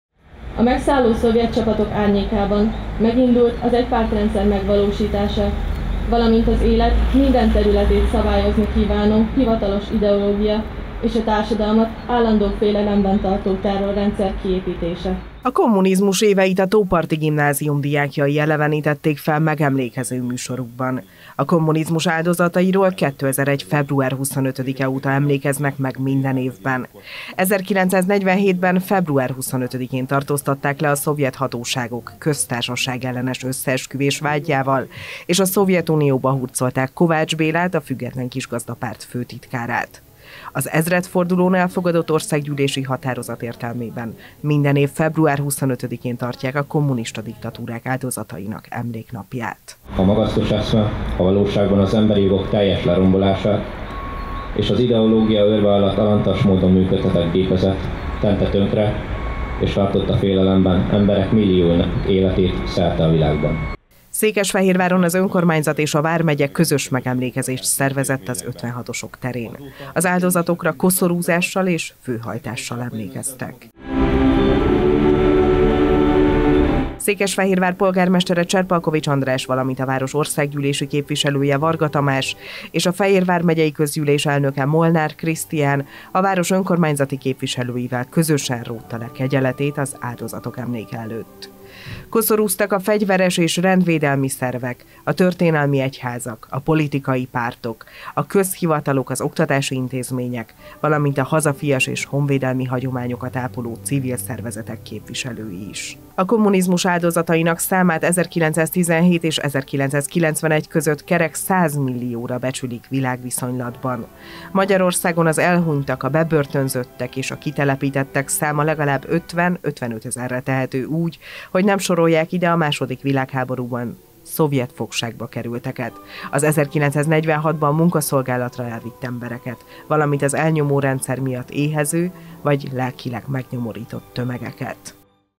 KOSZORÚZÁSSAL EMLÉKEZTEK A KOMMUNIZMUS ÁLDOZATAIRA
A kommunista vészkorszak áldozatairól 2001. február 25-e óta emlékeznek meg minden évben. 1947-ben február 25-én tartóztatták le a szovjet hatóságok köztársaság-ellenes összeesküvés vádjával és a Szovjetunióba hurcolták Kovács Bélát, a Független Kisgazdapárt főtitkárát. Székesfehérváron az önkormányzat és a vármegye közös megemlékezést szervezett az 56-osok terén.